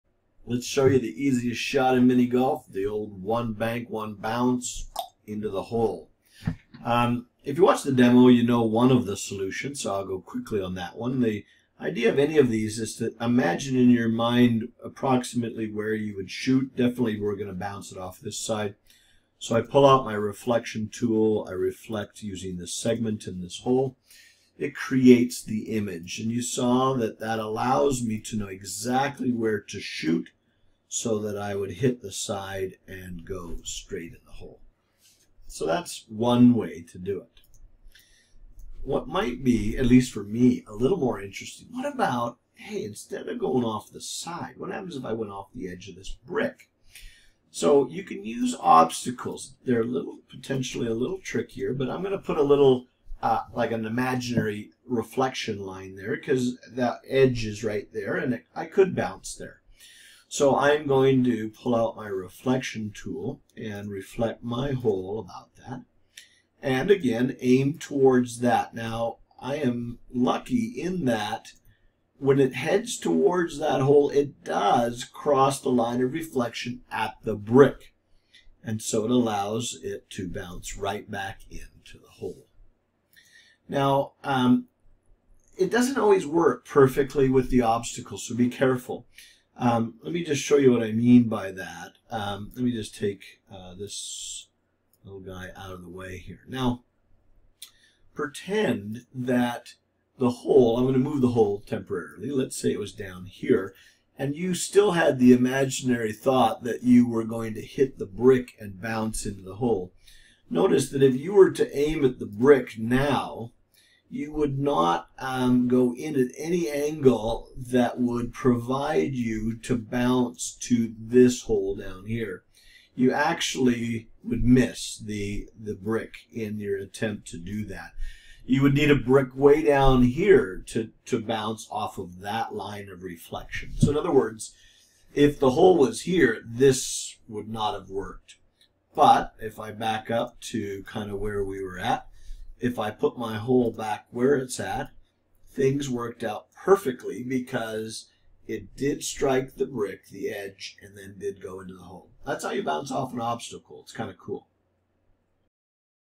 GeoGebra - G-CO.A.5.1 - One Bank
This assignment is designed to have students connect reflections to the applied game of mini-golf.